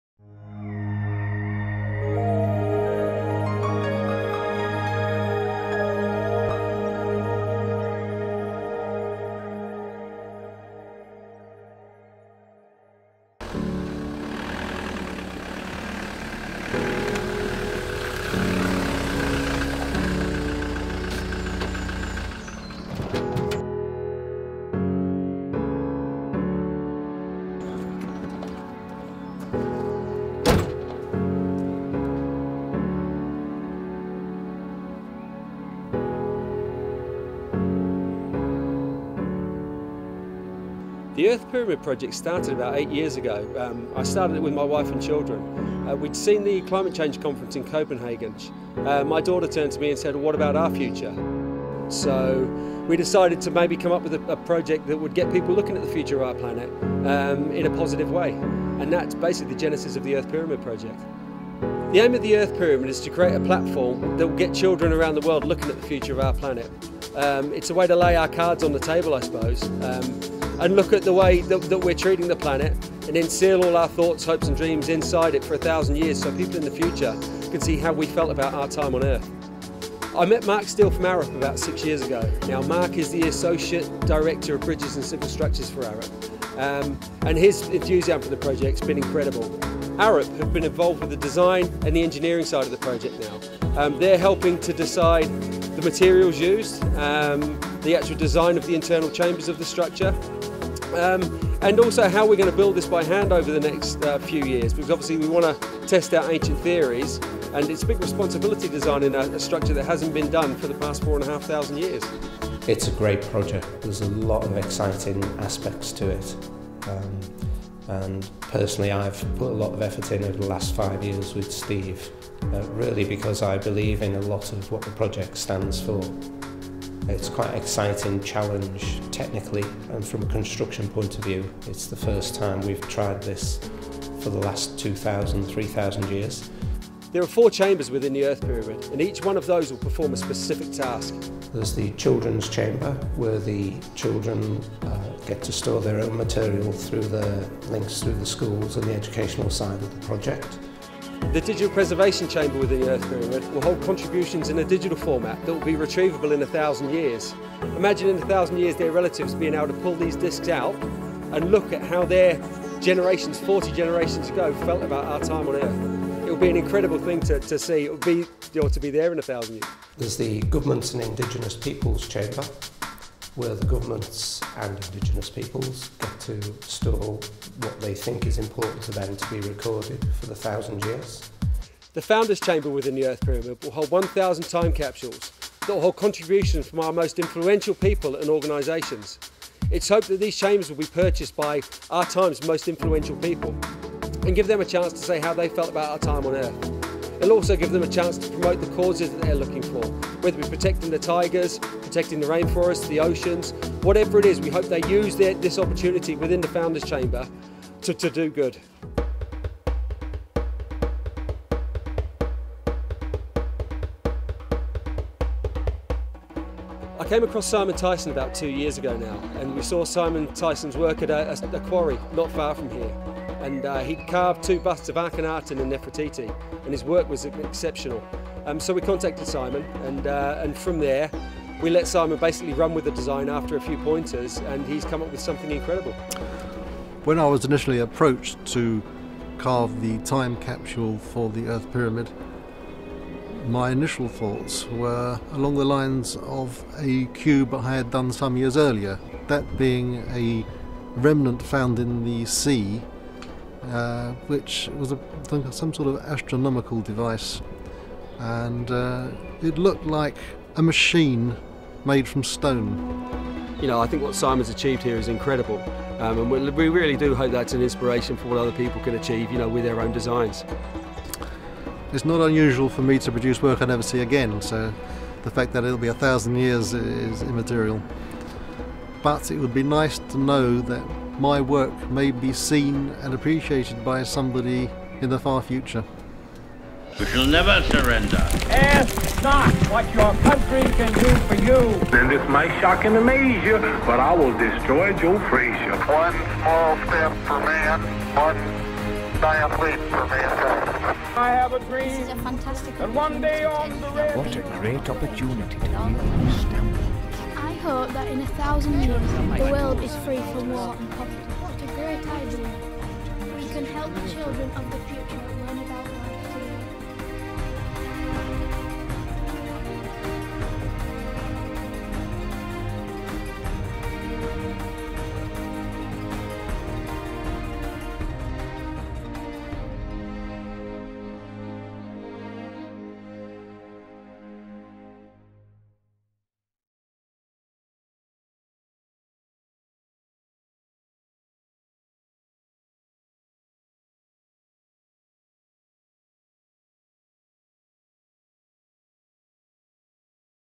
Inside the Earth Pyramid Project
Steve Ward explains how the Earth Pyramid project began, what the aims of the project are, and what is planned for the future of the Earth Pyramid project.

Mark Steele from ARUP talks about the technical challenges and the construction and together Mark & Steve together describe the chambers within the pyramid.

Steve introduces stone sculpture Simon Tyson, who talks about the time capsule's design.
Video produced by Moving Eye Productions.

00:40 - Steve talks about the background of the Earth Pyramid Project
01:20 - Mark Steele from ARUP contribution to the project
02:17 - Steve and Mark explain the four chambers of the pyramid:
02:21 - - Childrens' Chamber
02:30 - - Digital Preservation Chamber
02:50 - - Governments' and Indigenous Peoples' chamber
03:01 - - Founders' Chamber
03:29 - Simon Tyson and the Earth Pyramid Time Capsule
04:46 - Animation of the internal structure of the pyramid

The Earth pyramid will be a peace and environmental education monument that every nation and an entire generation of the worlds children can contribute to.